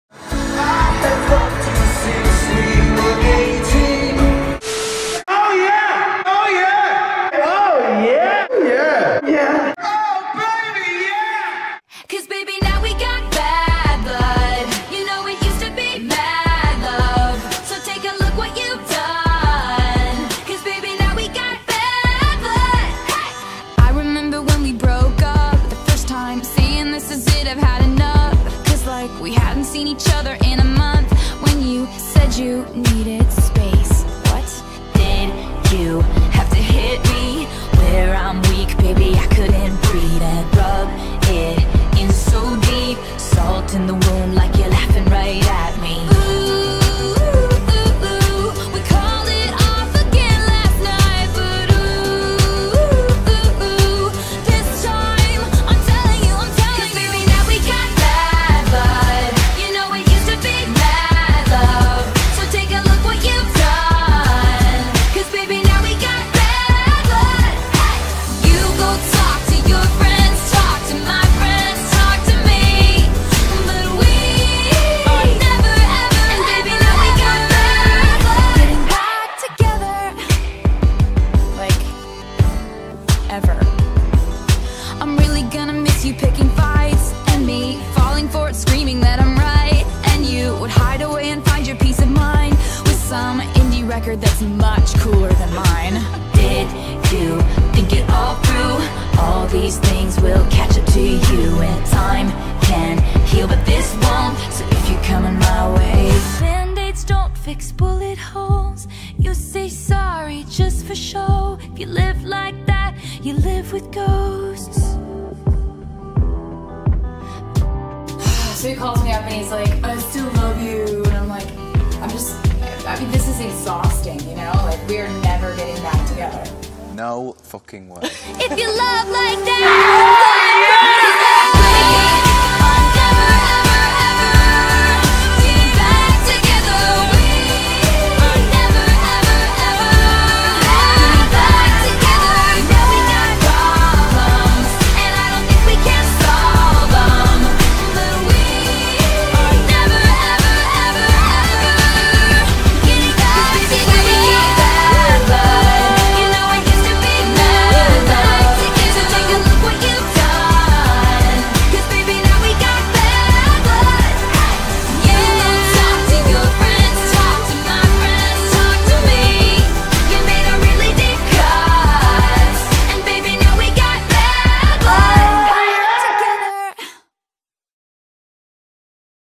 "OH..BABY, YEAH!" - Harry Styles.
ARGHHH exams are stressing me out so ((because you always tell me that I make you cry)) I decided to make this video bc I needed like. A MOTIVATION. TO. KEEP LIVING. LIKE. WHEN HARRY MOANS. MY DAYS GET BRIGHTER :) :) :)
Please give this video a THUMB UP if you like it! pleaseeee x.
I hope you like it babeeeees, love you all.  ♥